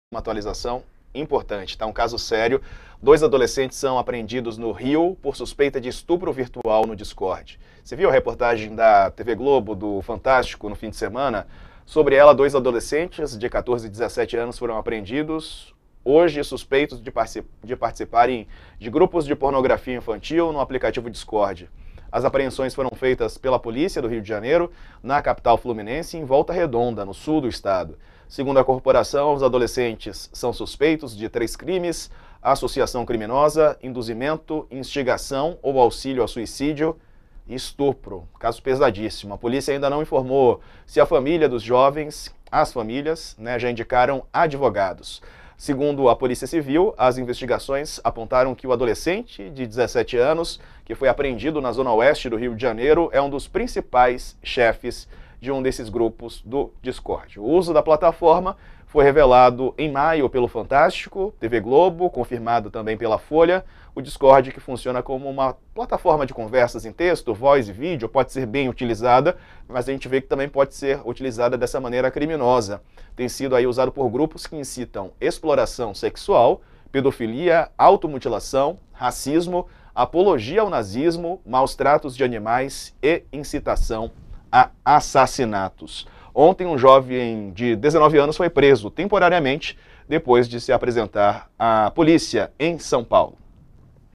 Discord: dois adolescentes são apreendidos no RJ por suspeita de estupro virtual na plataforma
Dois adolescentes, de 14 e 17 anos, foram apreendidos nesta terça-feira (27) suspeitos de participarem de grupos de pornografia infantil no aplicativo Discord. As apreensões foram feitas pela Polícia Civil do Rio de Janeiro, na capital e em Volta Redonda, no sul fluminense.